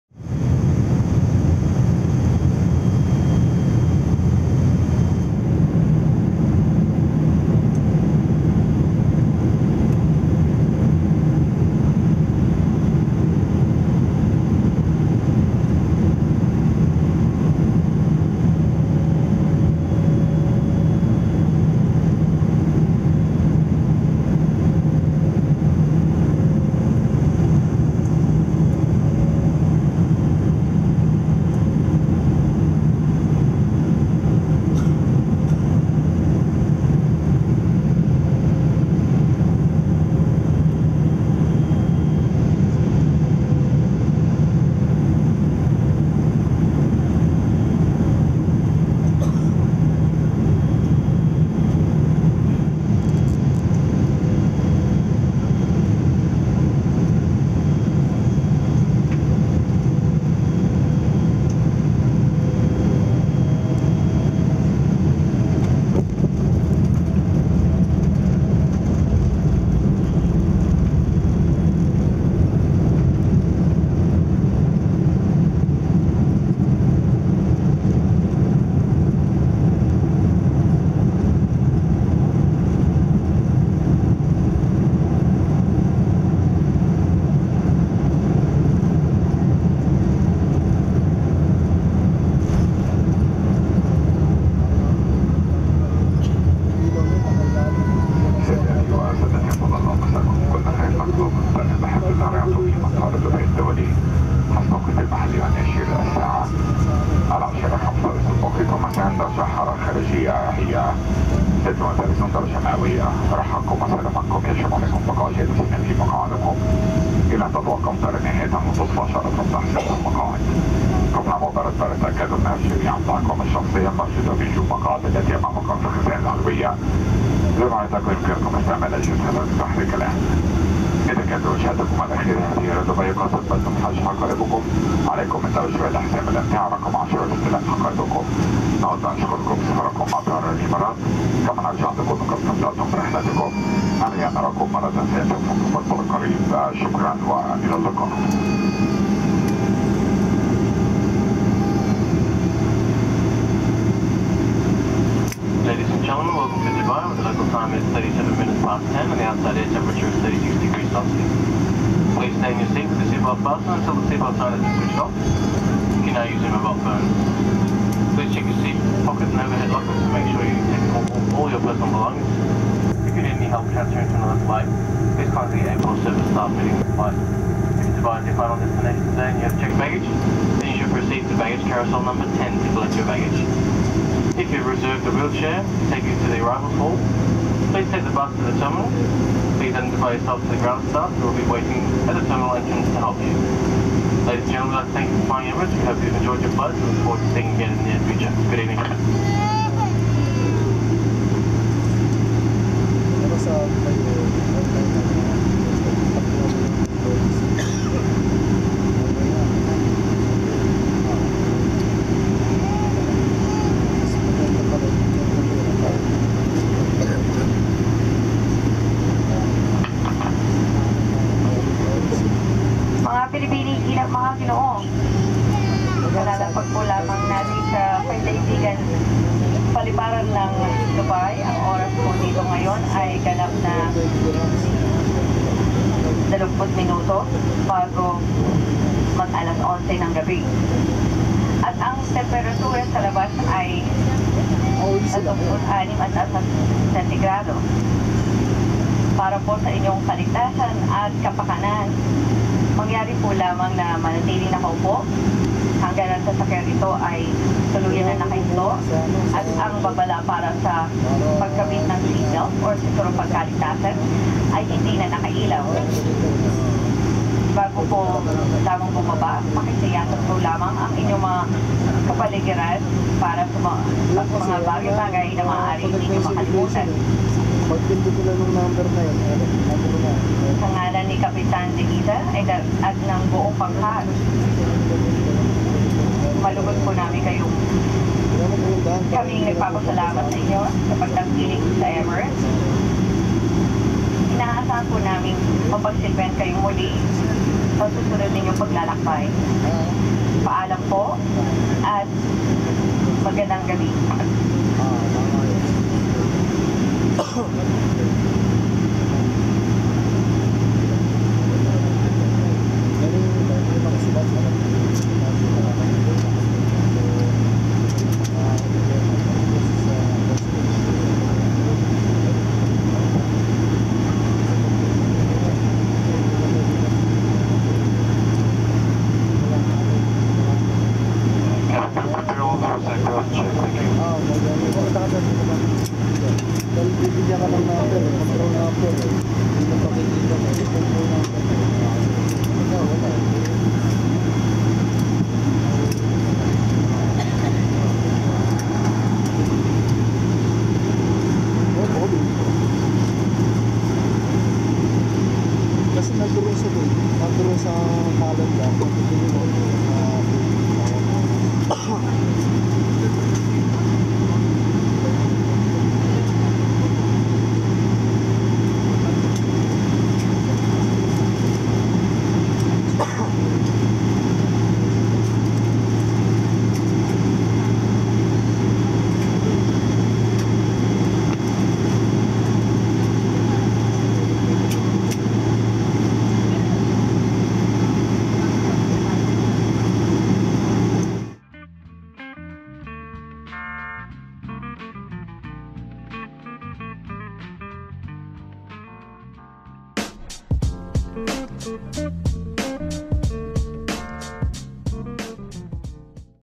Emirates B777-300ER LANDING in Dubai [EK333 | 2016]
Welcome aboard AviationStudio! Watch the Emirates B77W Landing in Dubai.

This video was shot last May 17, 2016. The flight was from Manila to Dubai with the flight number EK333 and with the aircraft registration of A6-EBL. For more information about this flight visit the trip report related with this trip.

Please note:
A6-EBL is now EI-GET and is with  Rossiya - Russian Airlines as of this year.